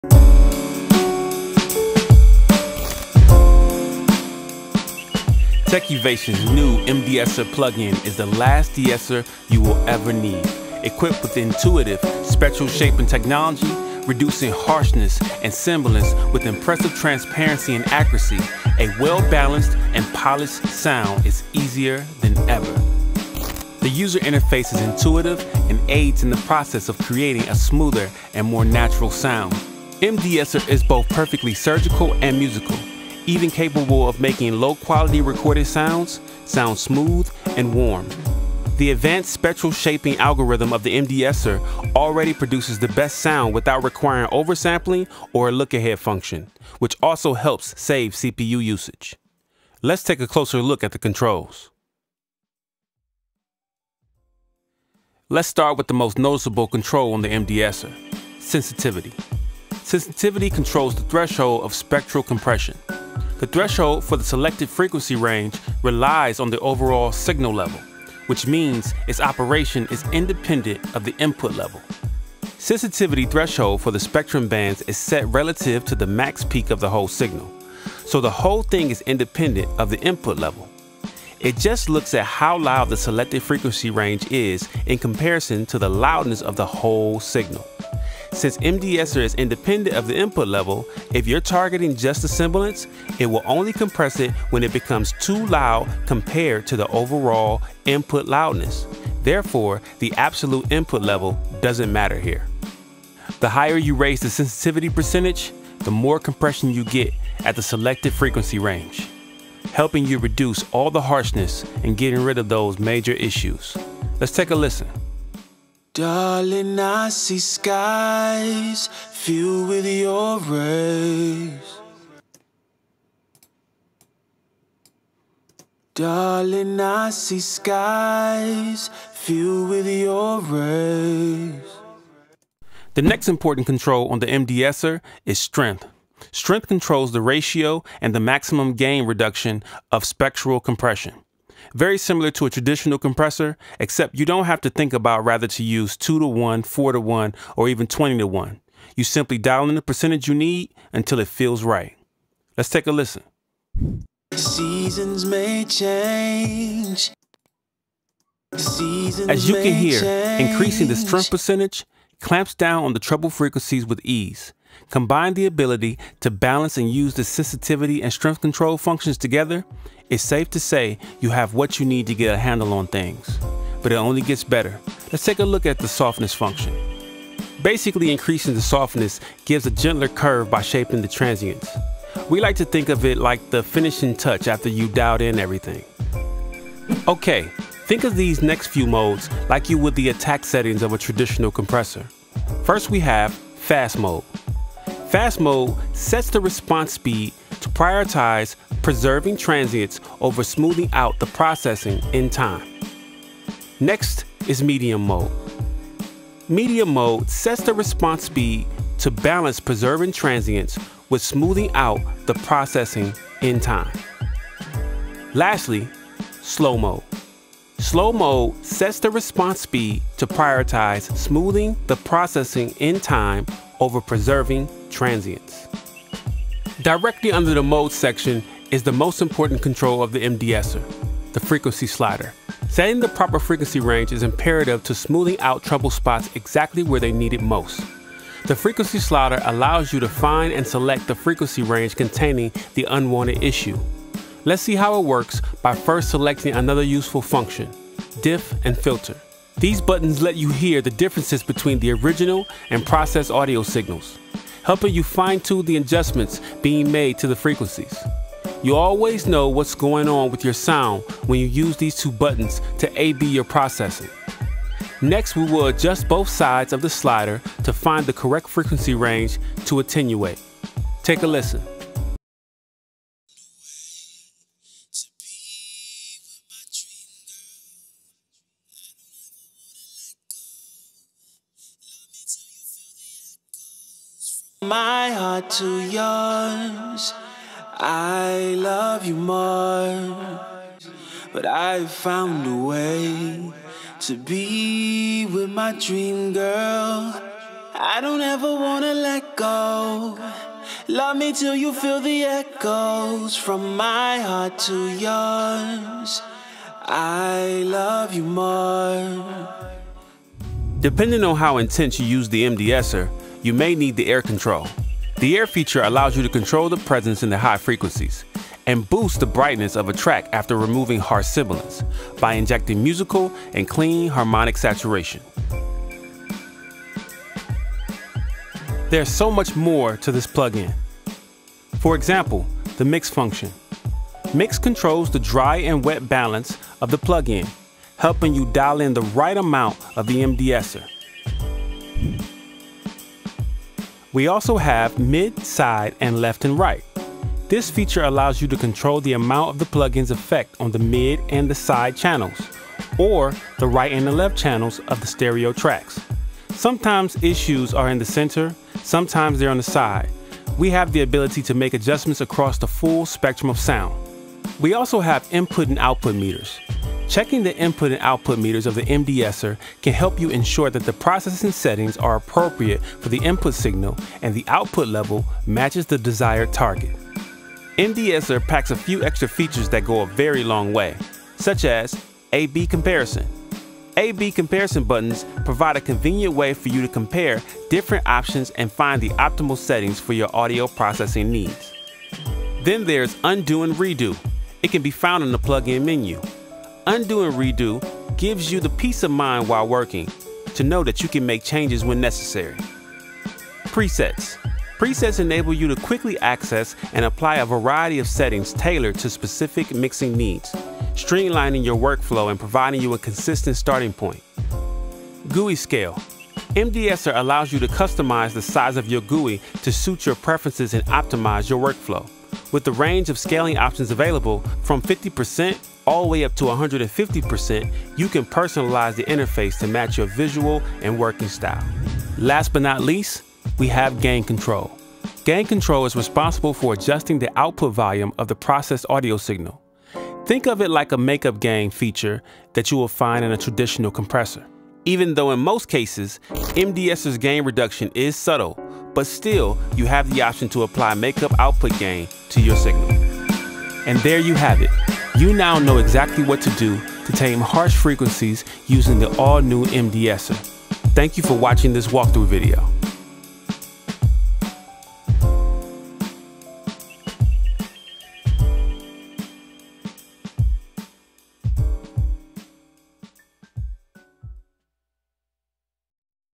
Techivation M-De-Esser Walkthrough
The complete Techivation M-De-Esser walkthrough and demo with explanations about its powerful features.